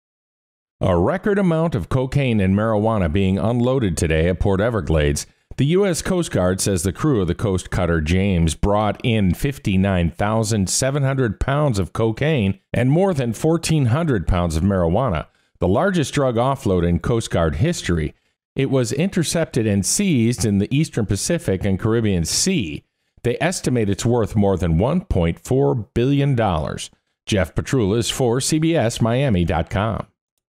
Coast Guard Offloads Record $1.4 Billion In Cocaine, Marijuana At Port Everglades
The U.S. Coast Guard is offloading a record-setting amount of cocaine and marijuana at Port Everglades on Thursday.